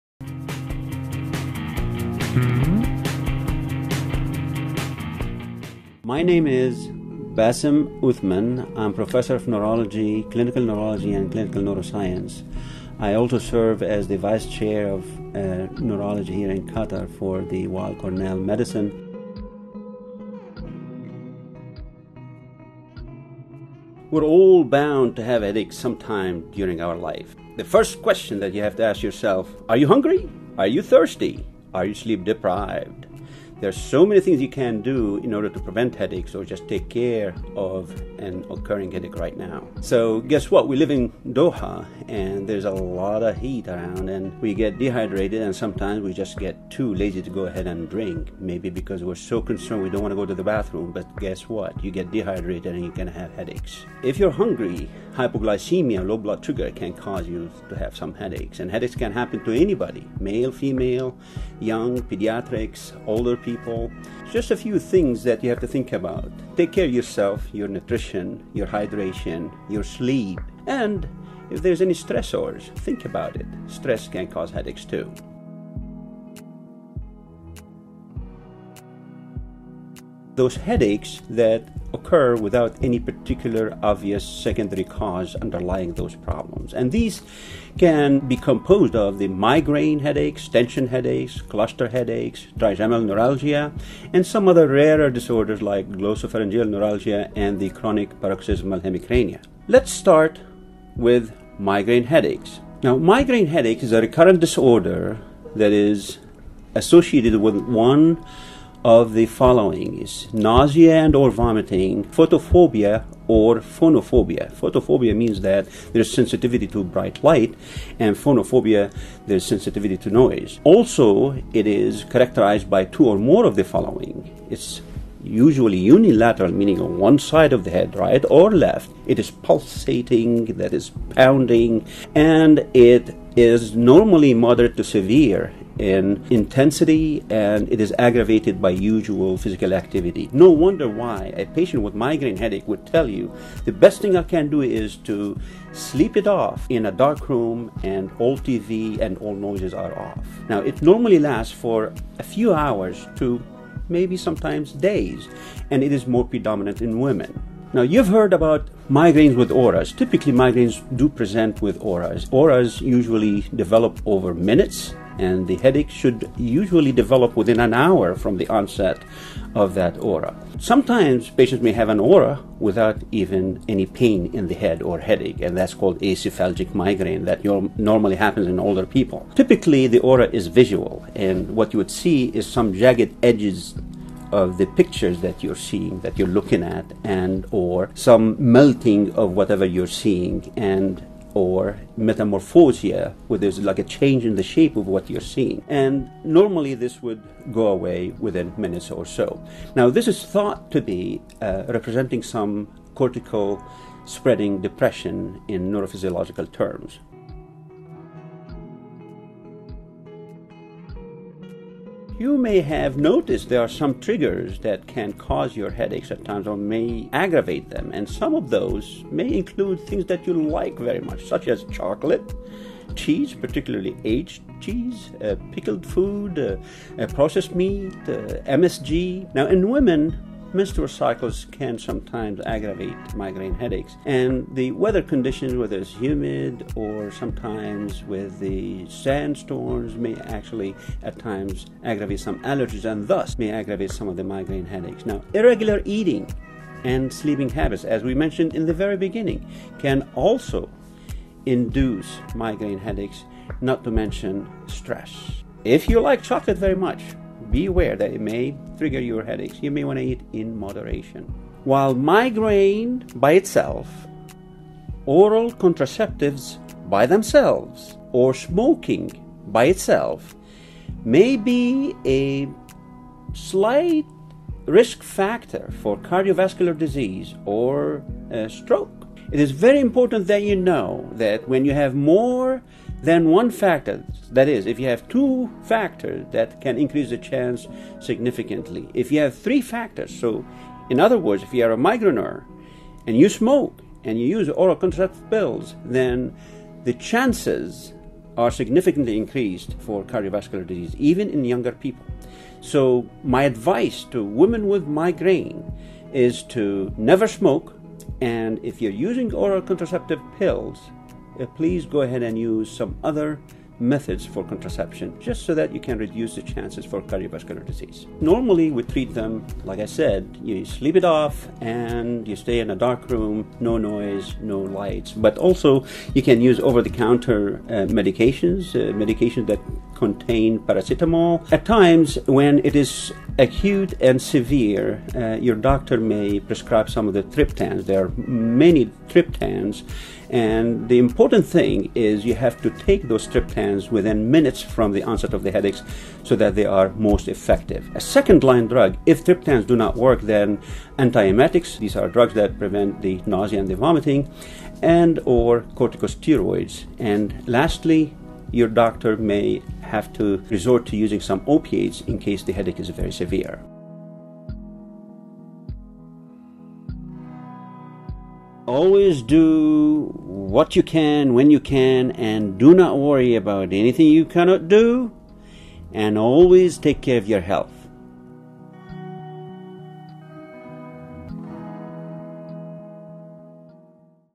How to Prevent Headaches - What's Up Doc? - Episode #13
Basim Uthman, MD
Vice Chair & Professor, Clinical Neurology
Professor of Clinical Neuroscience Director, Neurology Clerkship
Weill Cornell Medicine-Qatar

For more about Sahtak Awalan and its strategic partners visit: SahtakAwalan.com